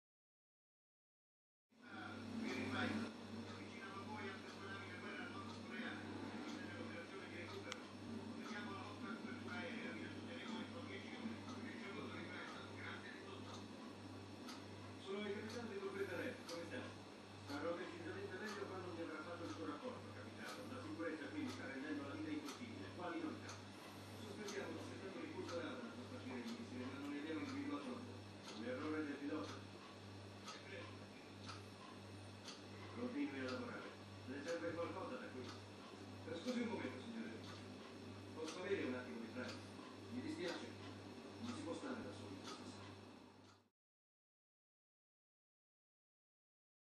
110130 codirosso spazzacamino
nuovo inquilino della mangiatoia, molto confidente. Notare la forma batuffolosa, che faccia freddo, fuori?